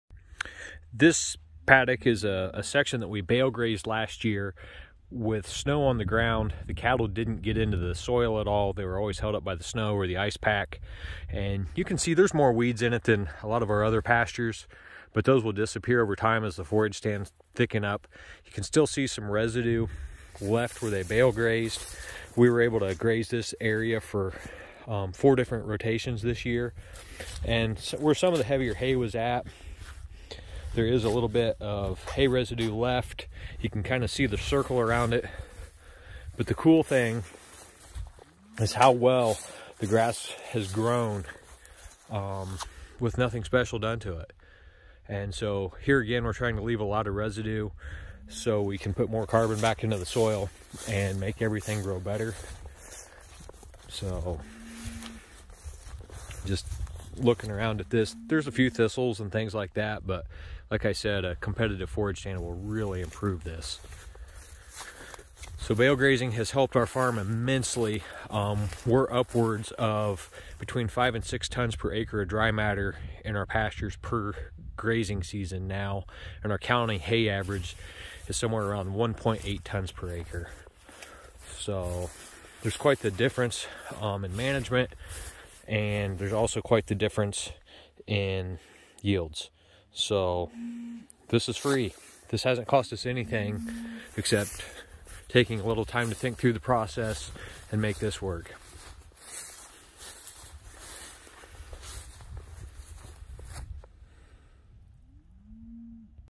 Evaluation of Bale Grazing on Pasture
Checking out a bale-grazed pasture for performance. Note that this pasture, along with others, is producing between 5-6 tons per acre - in a county that averages 1.8 tons per acre on their hay ground.